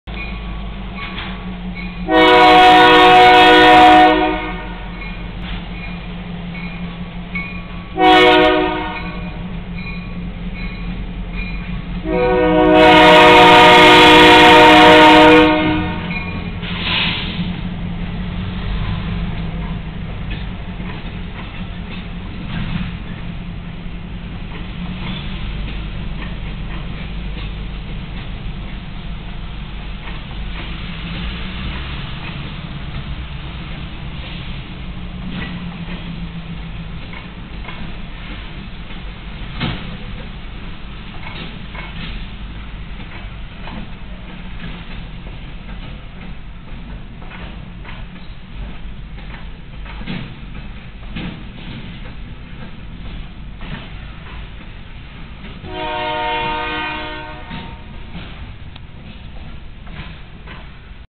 GMTX 2138 Brings 7 cars through Downtown Jefferson WI 9/24/14
GMTX 2138 brings 7 boxcars through Jefferson WI